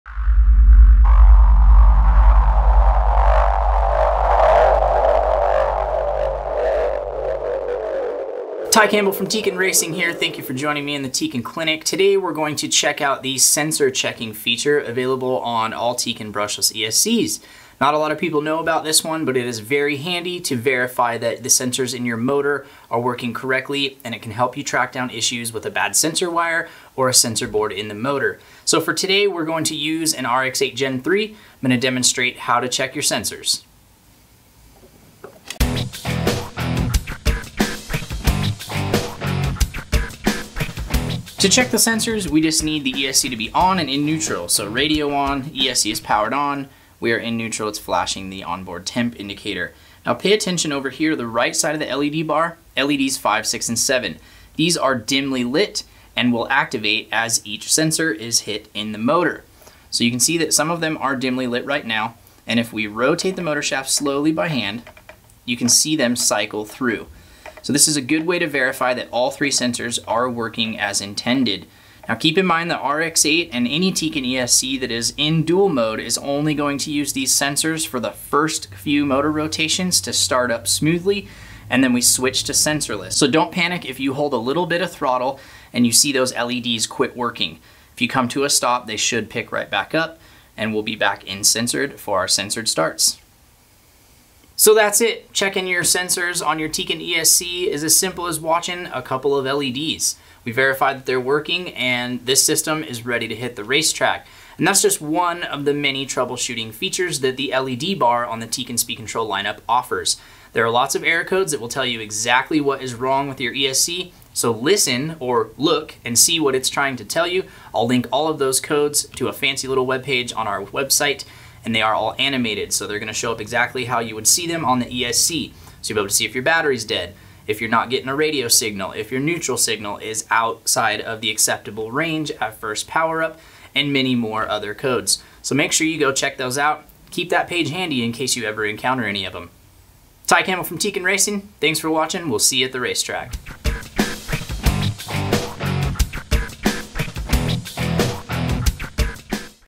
Sensor Checker | Tekin ESC Programming
Verify your sensors are working properly quickly and easily on your Tekin brushless ESC! If you ever feel like the ESC is running in sensorless, you can quickly check the sensors using the LED bar. With the ESC powered on and sitting in neutral, watch LEDs 5, 6 & 7 at the right hand side and each will light up as each sensor is hit.